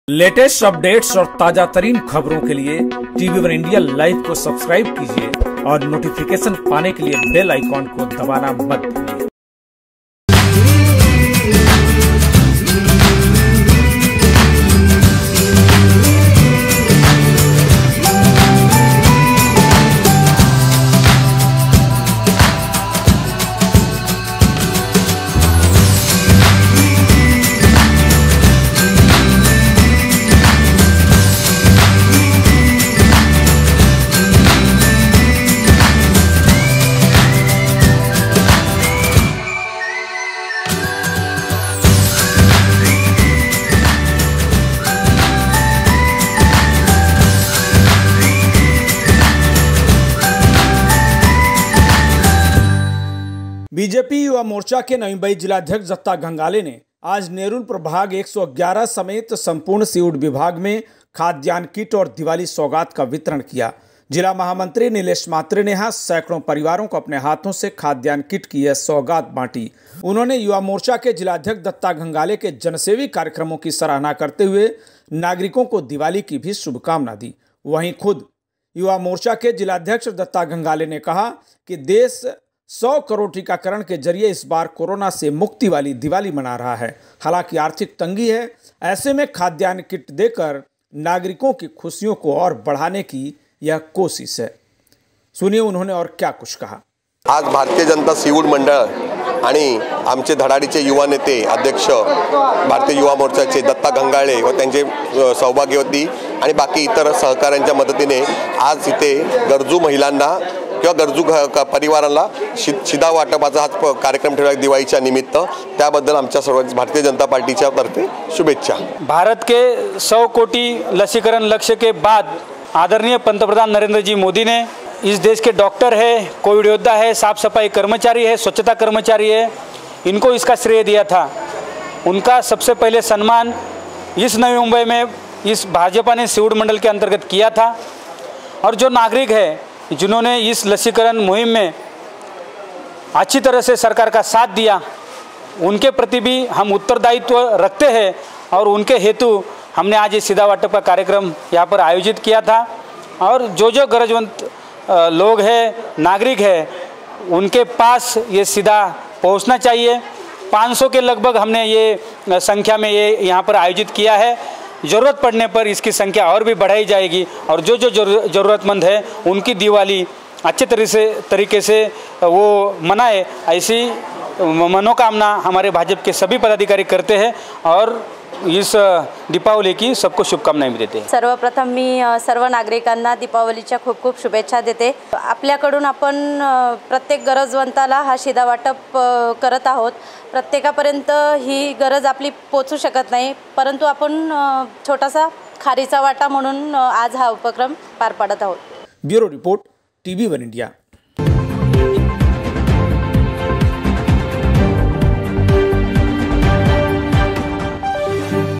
Datta Ghangale ने सीवुड इलाके में बांटा दिवाली सौगात | हजारों परिवारों को मिला खाद्यान्न किट
नवी मुंबई बीजेपी युवामोर्चा के जिलाध्यक्ष  दत्ता घंगाले ने आज सीवुड प्रभाग 111 में नागरिकों को दिवाली की सौगात के तौर पर खाद्यान्न किट का वितरण किया. इस दौरान बीजेपी के जिला महामंत्री निलेश म्हात्रे खास तौर पर मौजूद थे . उन्होंने युवा मोर्चा जिलाध्यक्ष दत्ता घंगाले के जनसेवी कार्यक्रमों की सराहना करते हुए इस राहत भरी सौगात बताया. वहीं खुद युवानेता दत्ता घंगाले ने कहा कि देश इस बार 100 करोड़ टीकाकरण के साथ कोरोना मुक्ति वाली दिवाली मना रहा है ऐसे में खाद्यान्न किट की यह छोटी सी मदद नागरिकों की खुशियों को और बढ़ाने की कोशिश है